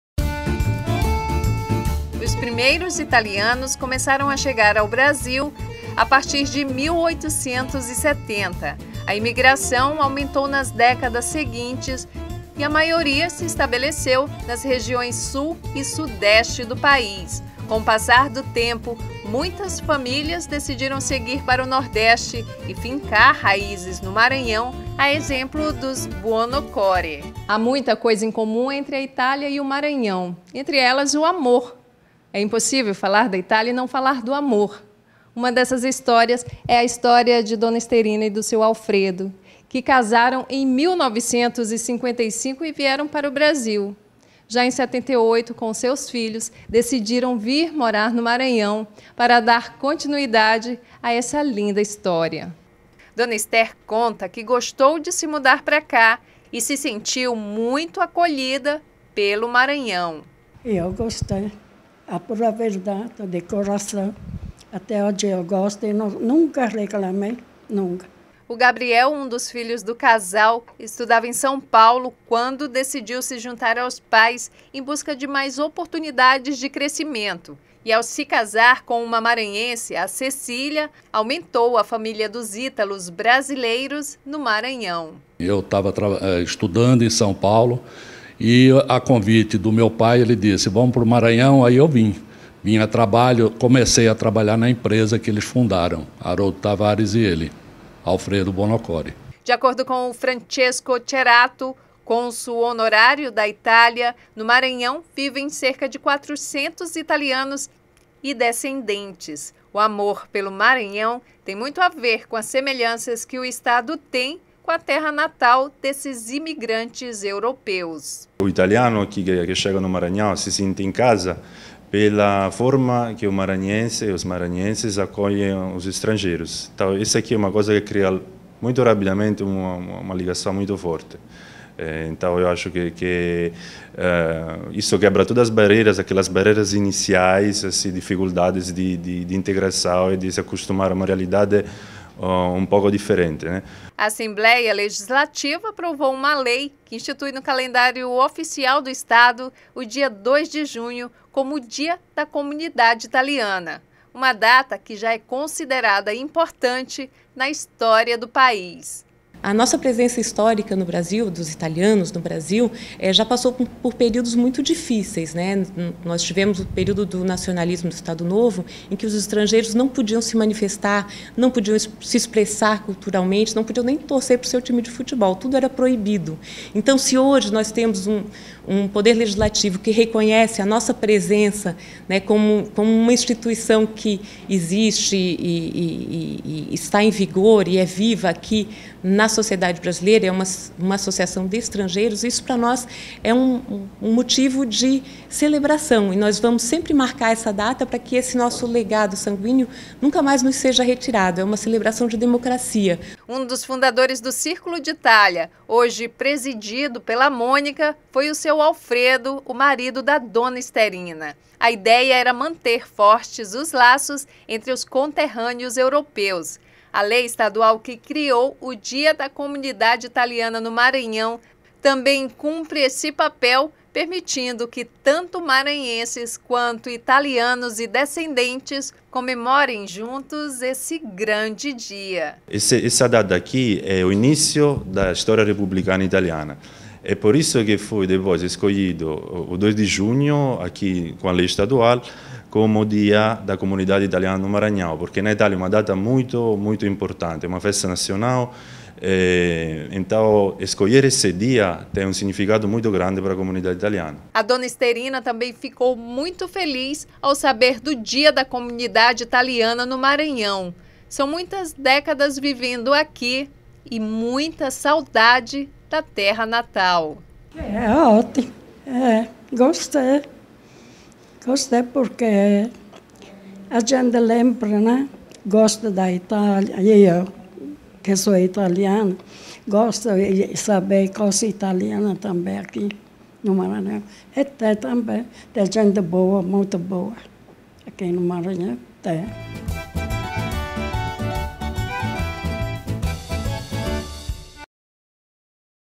Conheça a importância do Dia da Comunidade Italiana no Maranhão
E de acordo com o consulado da Itália no Maranhão, cerca de quatrocentos italianos vivem no Estado. Em homenagem a essa comunidade a Assembleia Legislativa aprovou a lei que institui no calendário oficial do Maranhão o dia 02 de junho como o Dia da Itália. Uma data que já é considerada importante na história do país.